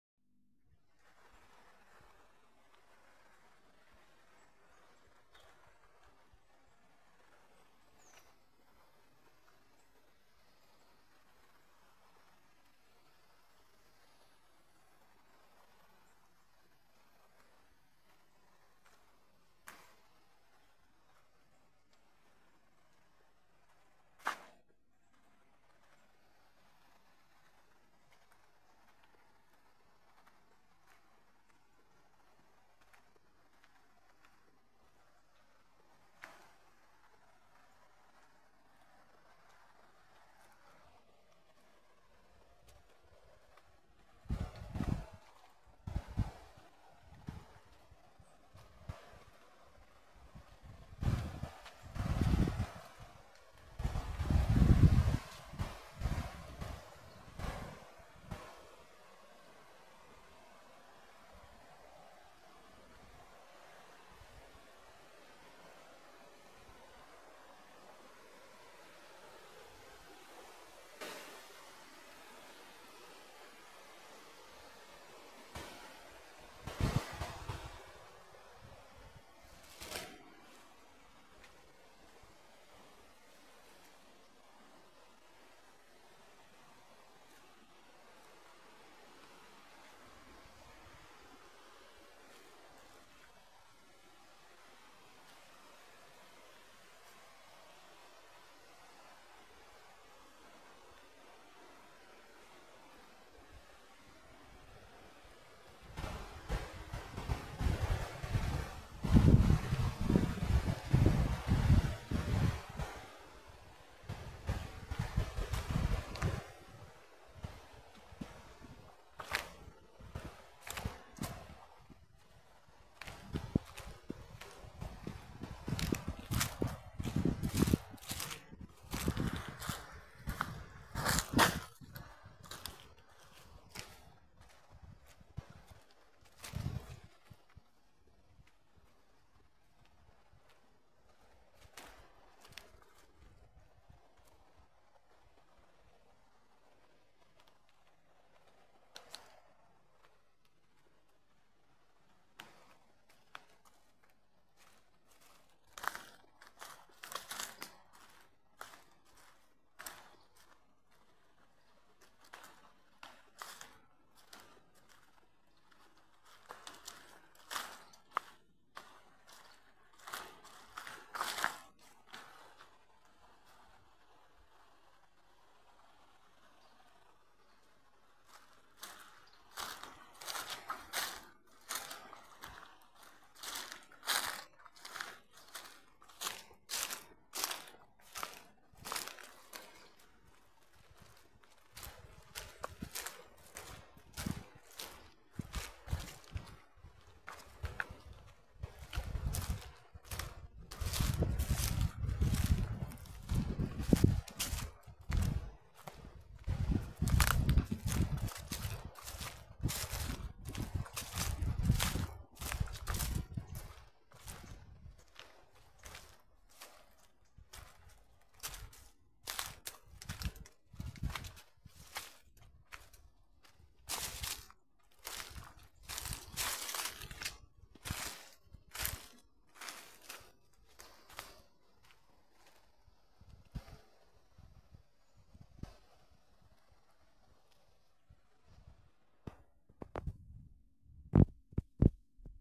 caminata en el rio barranca
es muy relajante caminar cerca de rios, da mucha paz